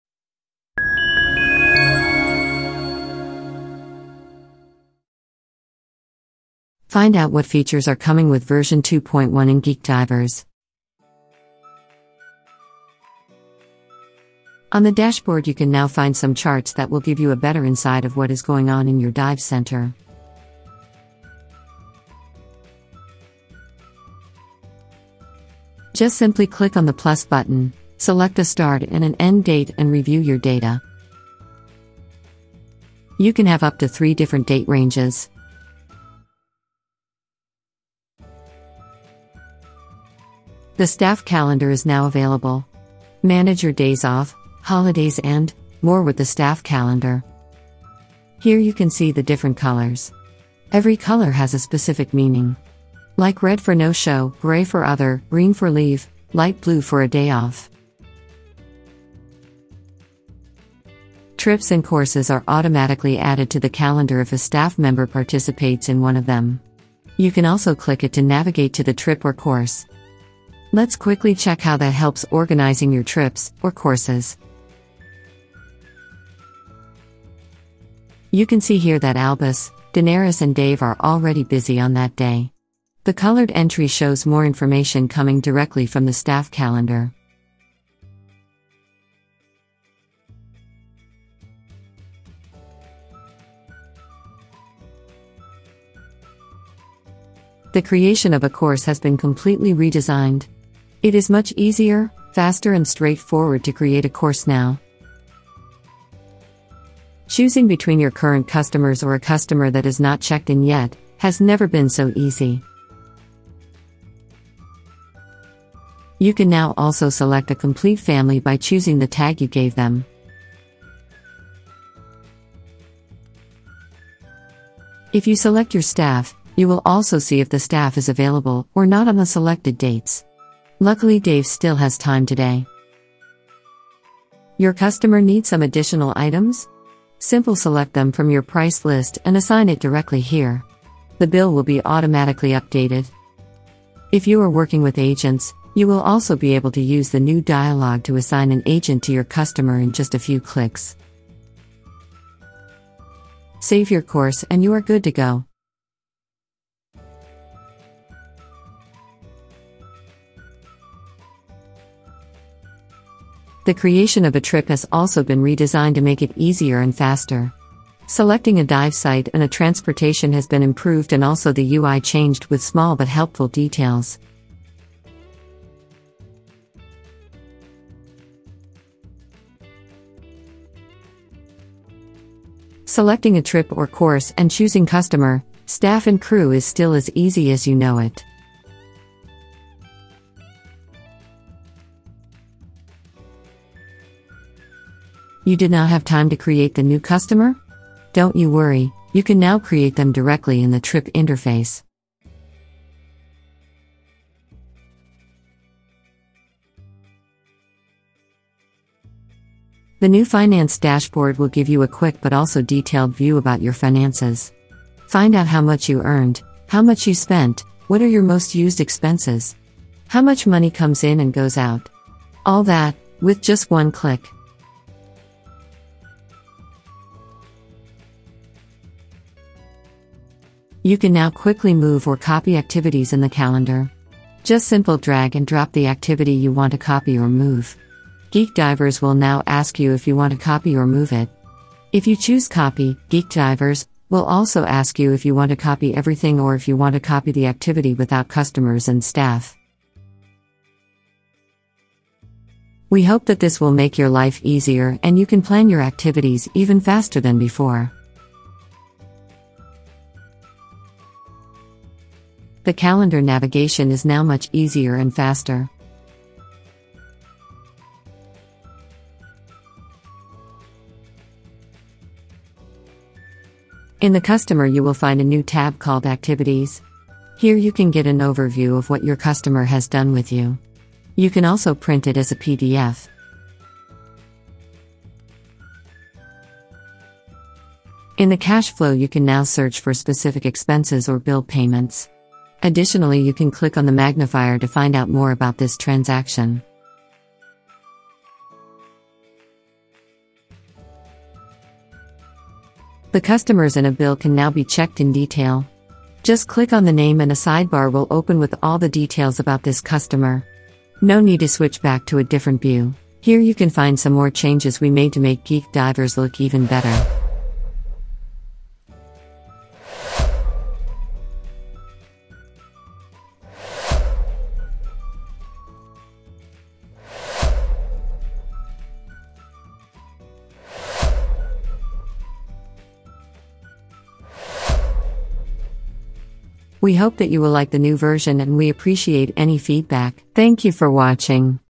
Geek Divers Update 2.1
Find out what´s new in Geek Divers Version 2.1
Content:

0:00 Intro
0:14 Dashboard
0:37 Staff Calendar
1:32 New Course Layout
2:36 New Trip Layout
3:26 New Financial Dashboard
3:48 Copy Activities
4:20 Calendar Navigation
4:33 Customer Activities
4:47 Search Cash Flow
5:02 Customer infos in bill
5:13 UI Changes